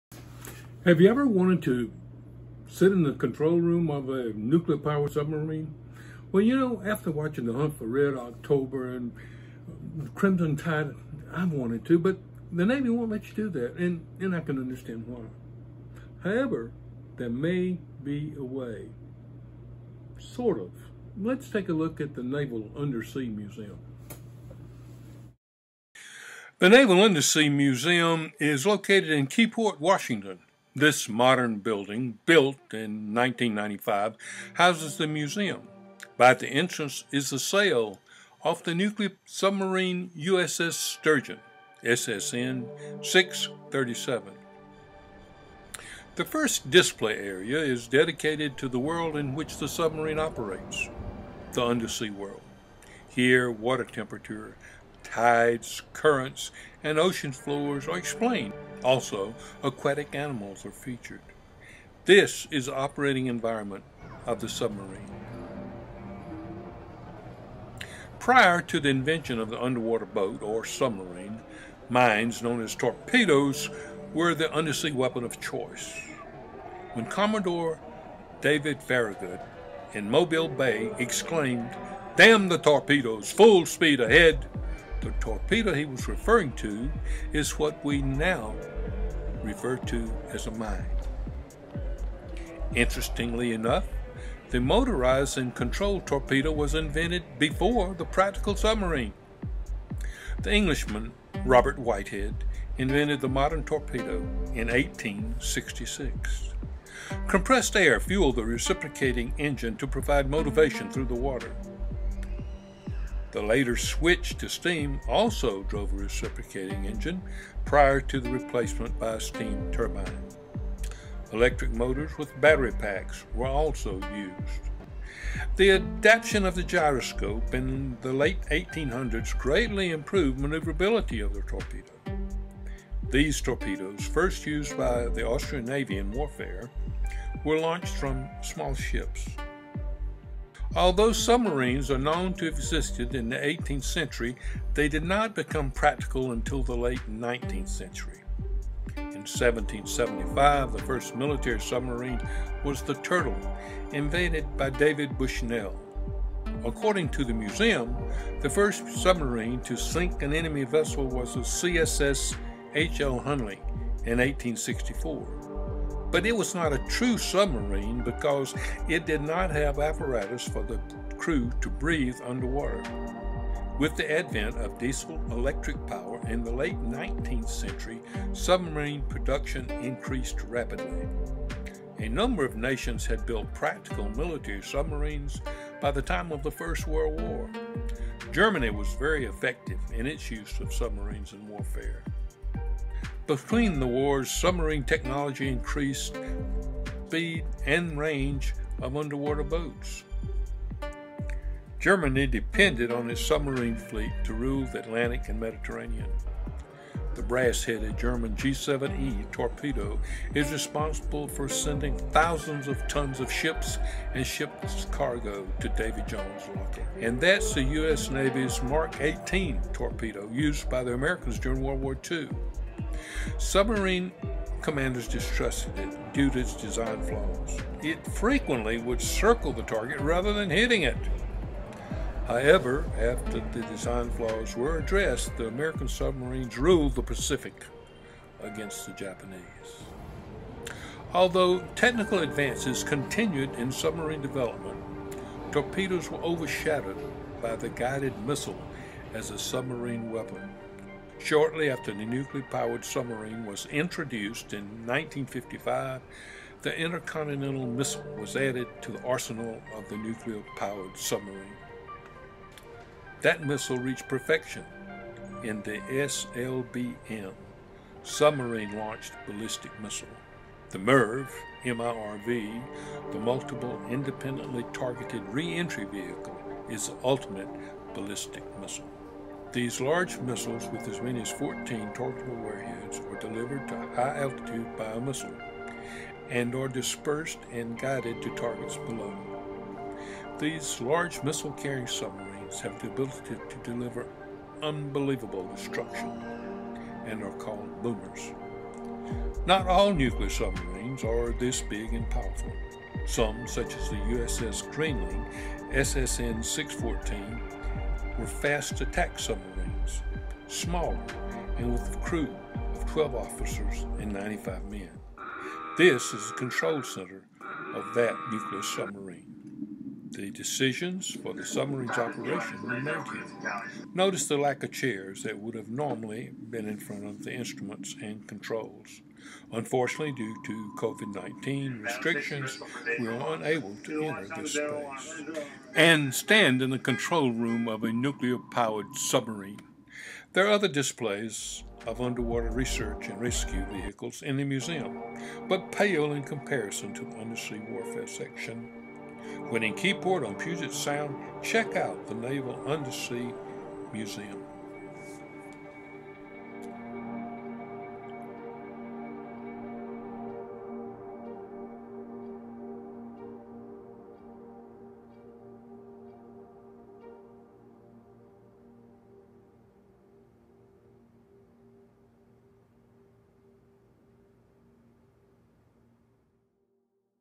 UNDERSEA WARFARE TECHNOLOGY
The video follows the evolution of undersea warfare technology from the inception to the modern day. Through static displays and actual weapons the development of the underwater boat, the submarine,  is chronicled. The weapons of the undersea vehicle of war is also traced from the explosive charges attached  the seafloor to the modern torpedo and intercontinental ballistic missile. Except for some archival film footage all video is shot at the Naval Undersea Museum in Keyport, Washington.

New drawings, photos and other stuff on Instagram @ ttexastony

Comments are greatly appreciated.